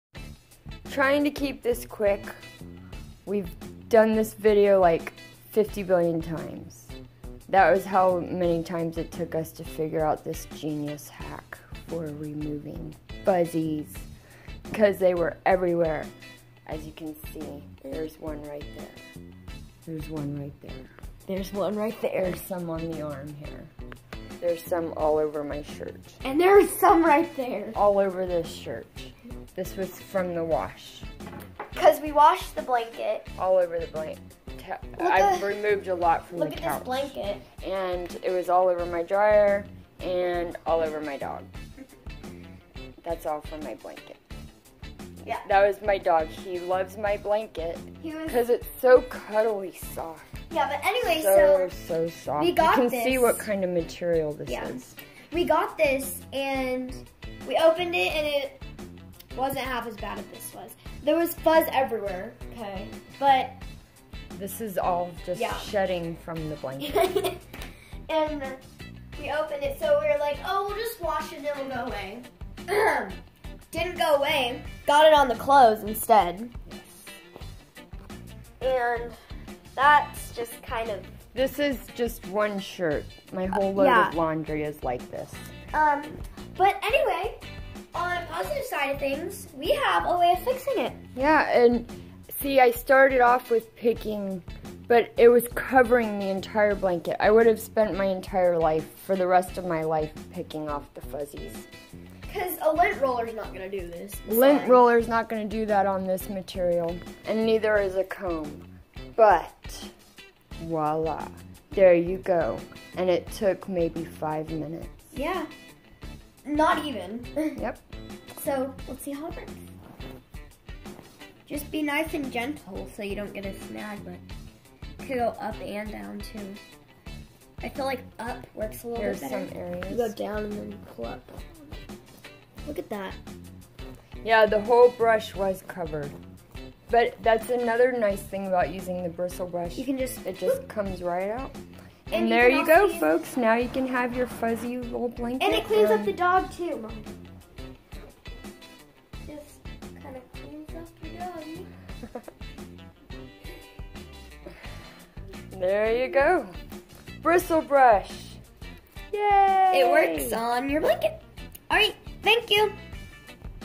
Fastest way to remove fuzz from a fluffy blanket
After trying different ways of removing the fuzz from this blanket we finally came up with this genius idea.